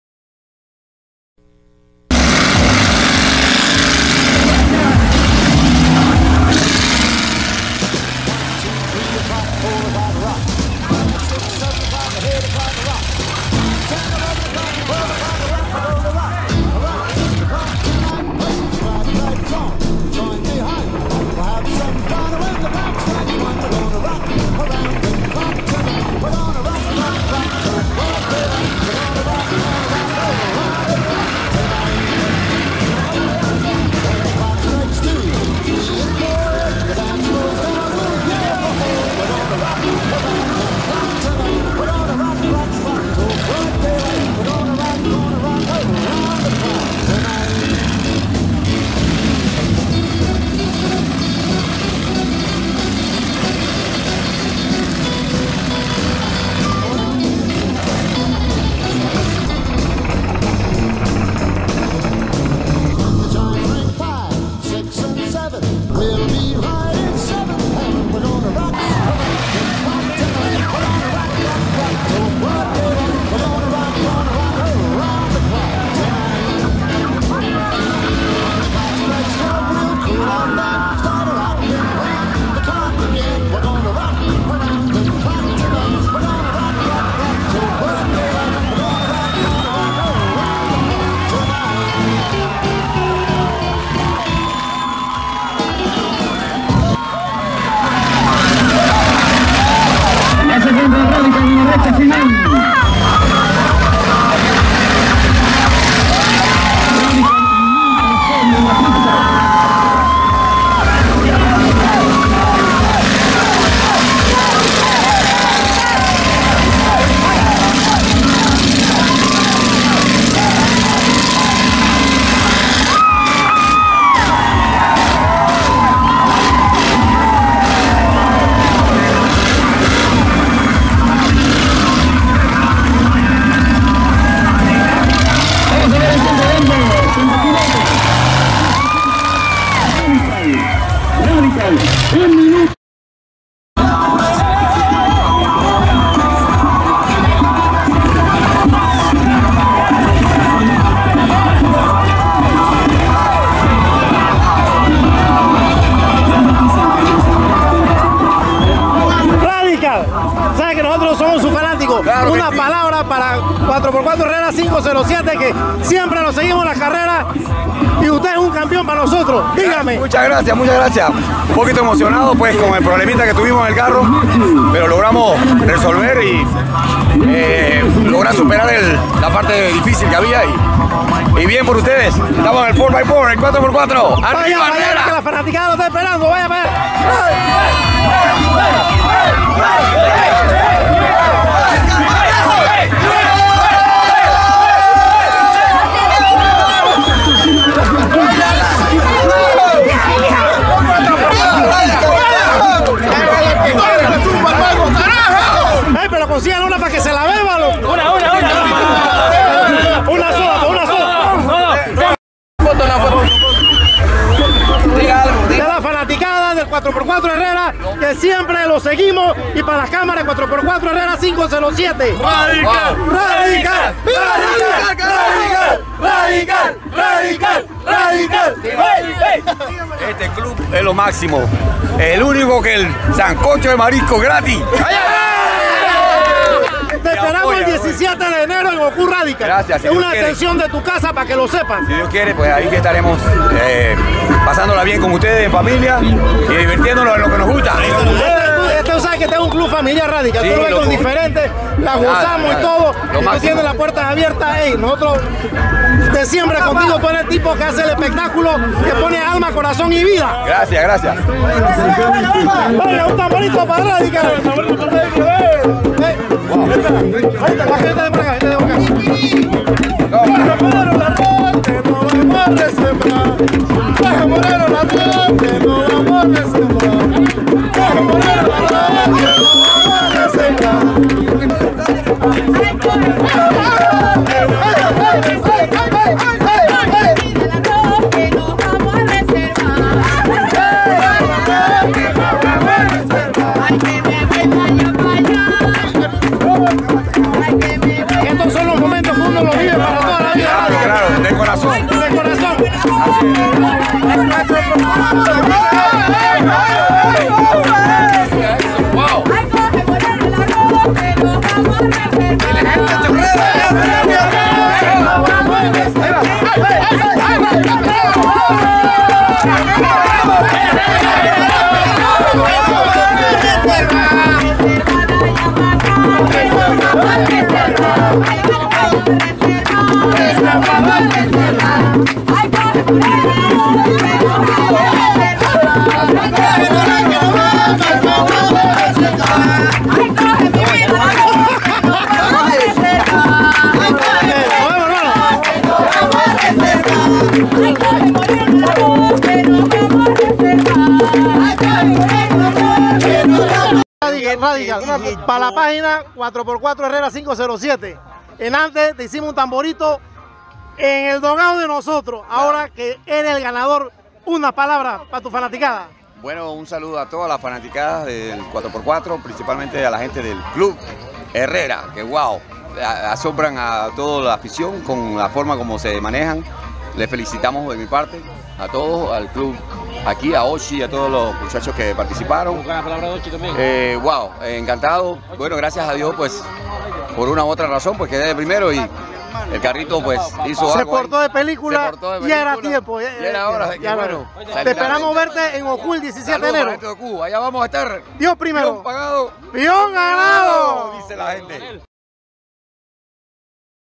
RADICAL GUARARE 13 DIC 2015
UN GUERRERO, UN CABALLERO. FELICIDADES PRIMER LUGAR 8 CILINDROS PISTA CHUMAJAL GUARARÉ 13 DICIEMBRE 2015